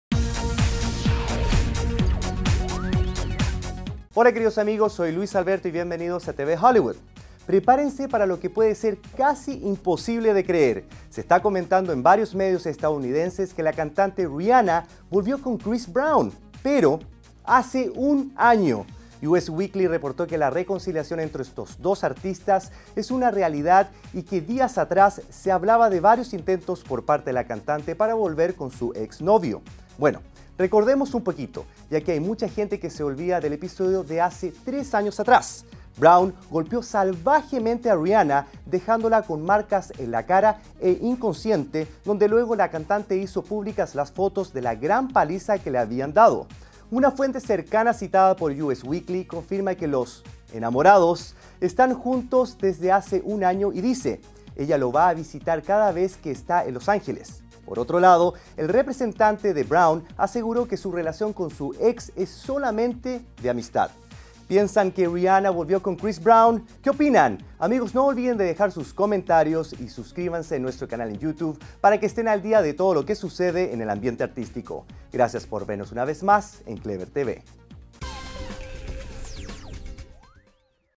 Rihanna volvió con Chris Brown ¡hace un año!
Hola queridos amigos soy Luis Alberto bienvenidos a TeVe Hollywood...Prepárense para lo que puede ser casi imposible de creer...se esta comentando en varios medios estadounidenses que la cantante "Rihanna" volvió con "Chris Brown"...pero... ¡hace un año!.. "Us Weekly" reportó que la reconciliación entre estos dos artistas es una realidad y  que días atrás, se hablaba de varios intentos por parte de la cantante para volver con su ex novio. Bueno, recordemos un poquito, ya que hay mucha gente se le olvida del episodio de hace tres años atrás: "Brown" golpeó salvajemente "Rihanna" dejándola con marcas en la cara e inconsciente, donde luego la cantante hizo públicas las fotos de la gran paliza que le habían dado. Una fuente cercana citada por "Us Weekly" confirma que los ¿enamorados? están juntos desde hace un año y dice: "Ella lo va a visitar cada vez que está en Los Ángeles",...Por otro lado el representante de "Brown" aseguró que su relación con su ex es solamente de amistad. Piensan que Rihanna volvió con Chris Brown? Que opinan? Gracias por vernos una vez más en Clevver TeVe